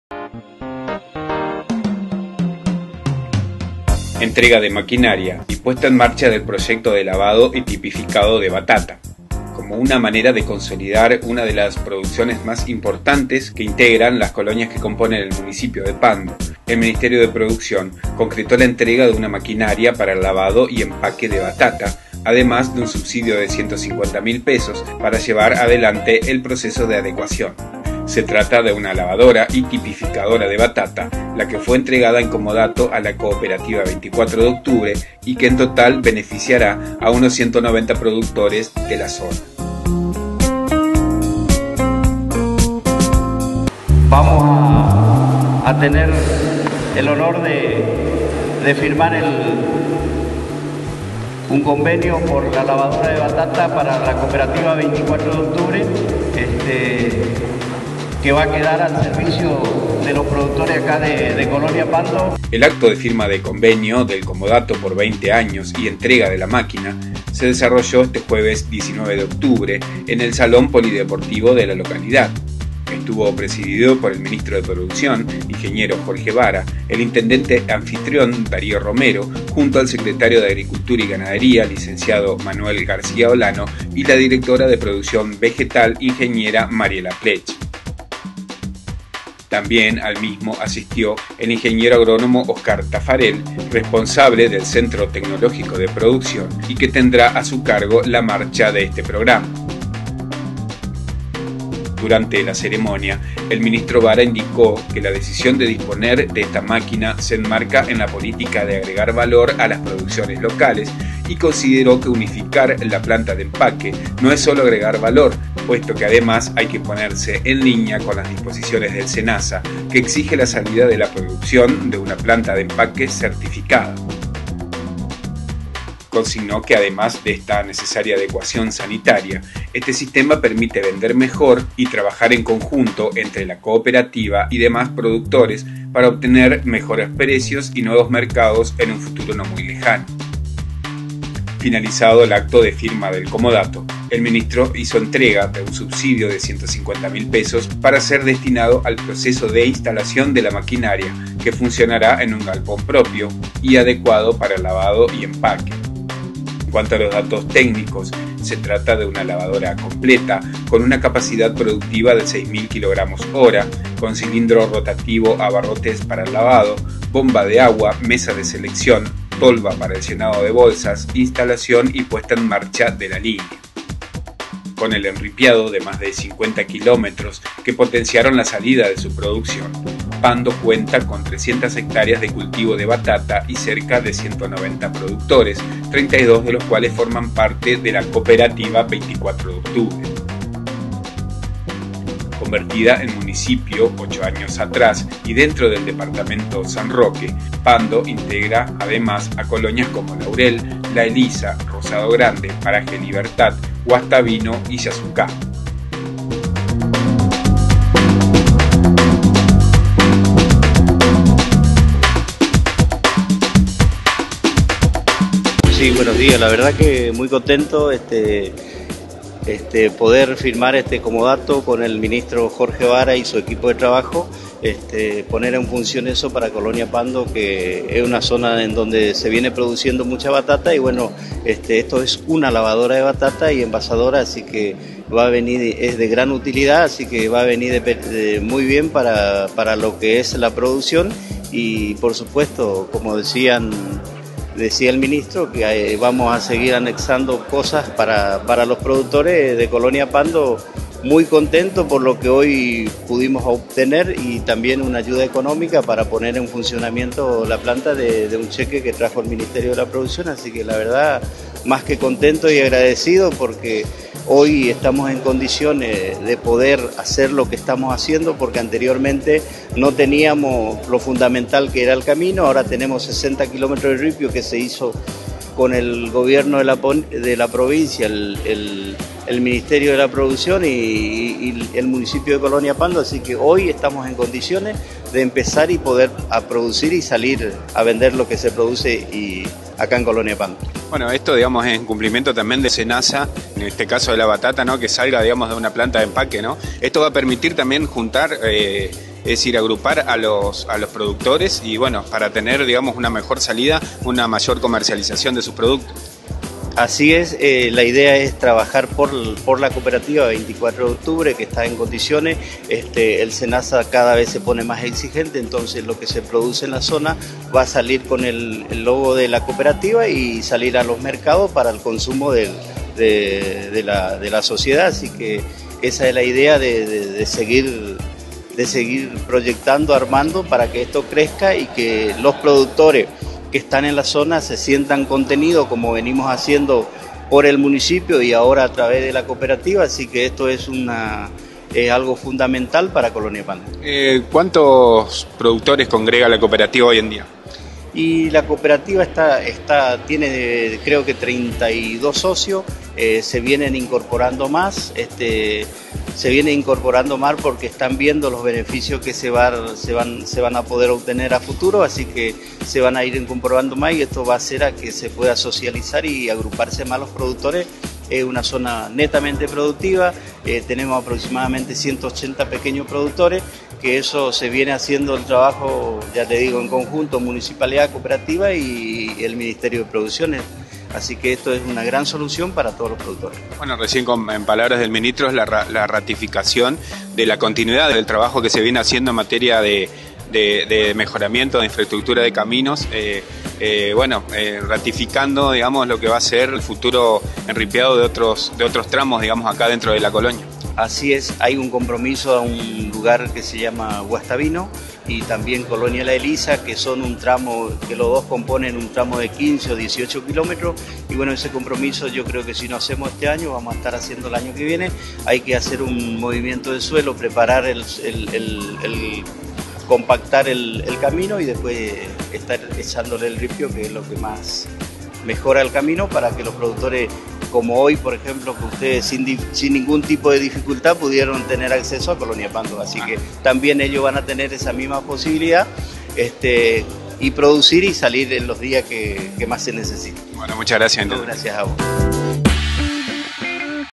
Proyecto Lavado y Tipificado de Batata en Pando:
Como una manera de consolidar una de las producciones más importantes que integran las colonias que componen el Municipio de Pando, el Ministerio de Producción concretó la entrega de una maquinaria para el lavado y empaque de batata, además de un subsidio de $ 150.000 para llevar adelante el proceso de adecuación. Se trata de una Lavadora y Tipificadora de Batata, la que fue entregada en comodato a la Cooperativa 24 de octubre y que en total beneficiará a unos 190 productores de la zona.
El acto de firma de convenio del comodato por 20 años y entrega de la máquina, desarrollado el jueves (19 de octubre) a media mañana en el Polideportivo de la localidad, estuvo presidido por el ministro de Producción, Ing. Jorge Vara, y el intendente anfitrión Darío Romero; junto al secretario de Agricultura y Ganadería, Lic. Manuel García Olano; la directora de Producción Vegetal, Ing. Agr. Mariela Pletsch, y el responsable del Centro Tecnológico de Producción (CETEPRO), Ing. Agr. Oscar Taffarel, que tendrá a su cargo la marcha del programa. También asistieron integrantes de la Cooperativa beneficiada, pequeños productores de las colonias, y público en general.